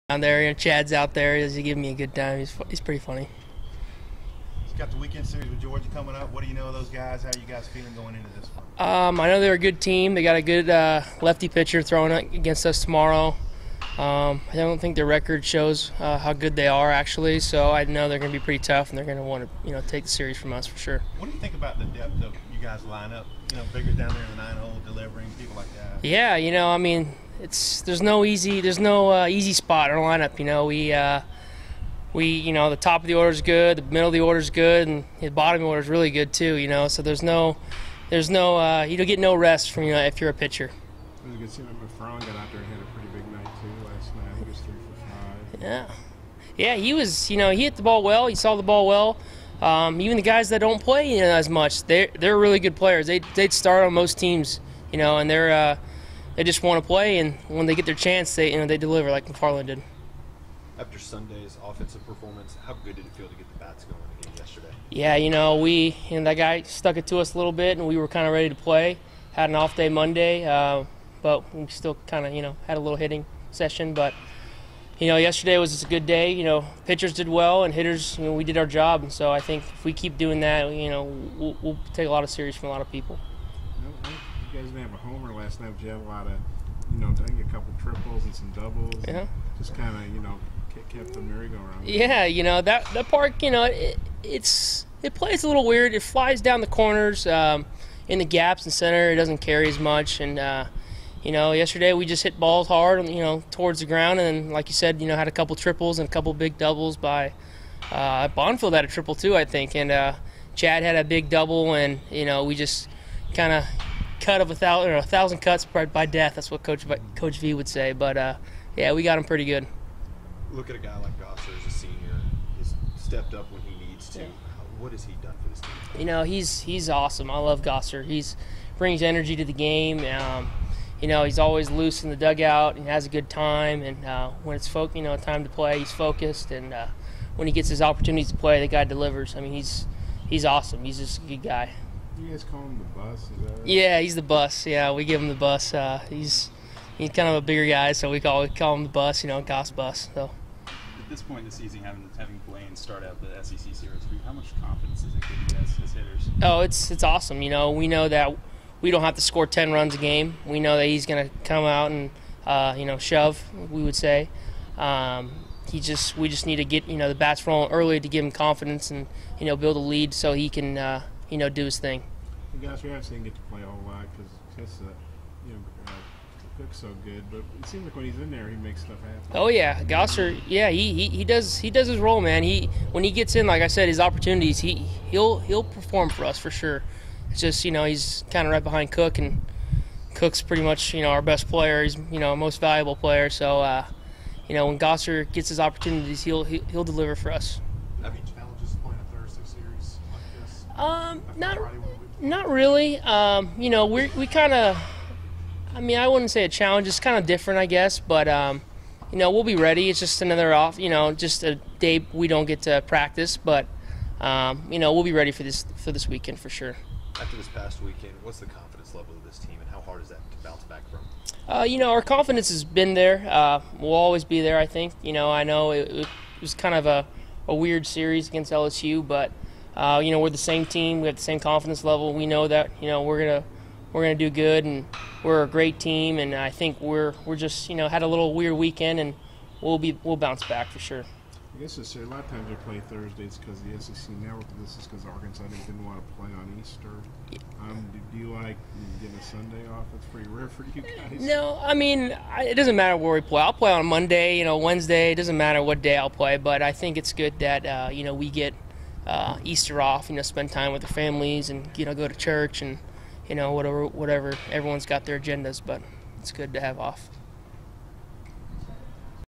Jake Arledge pre-practice April 12
Jake Arledge on bouncing back from LSU against Missouri State and more